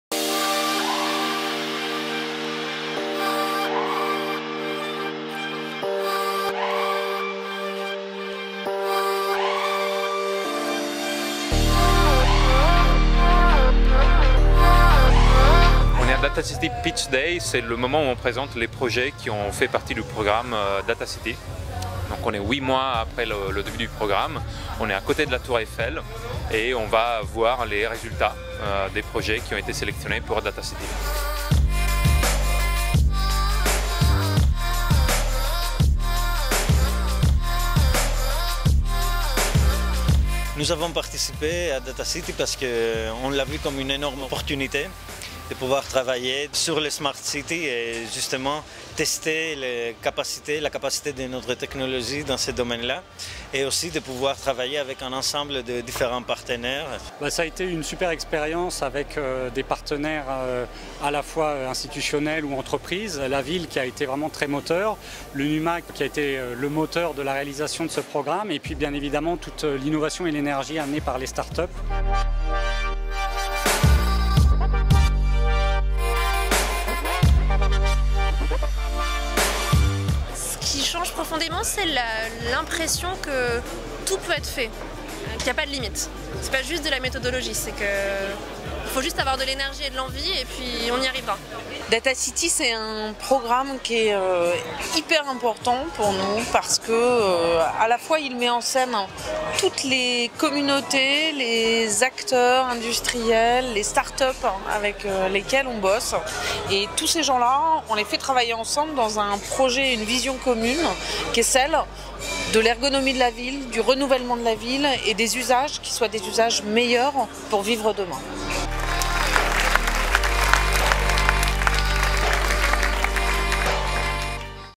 [DATACITY] - SAISON 1 - PITCH DAY
La première édition de DataCity est désormais terminée. Après 8 mois de consultations, de tests et de collaborations, ce programme d'innovation s'est achevé par la présentation des expérimentations : le Pitch Day. DataCity a fait émerger des alliances inédites entre startups et industriels. Retour sur ce projet qui veut révolutionner la ville.